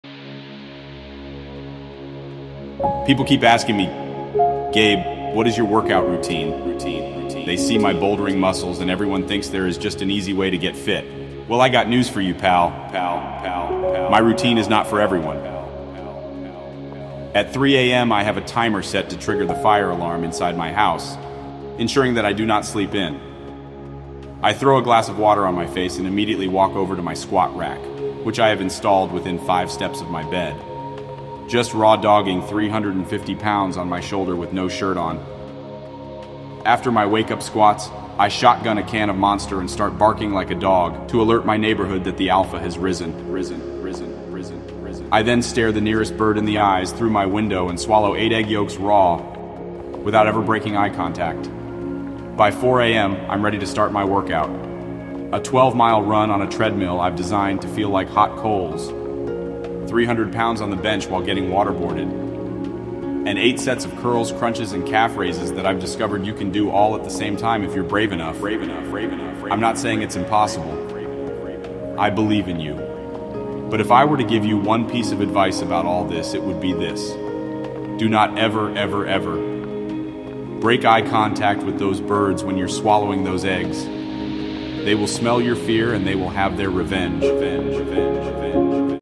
AI Diaries: Gabe Kapler shares his workout routine on Opening Day
I recreated San Francisco Giants manager Gabe Kapler's diary entries using artificial intelligence. This entry is about his elusive workout routine.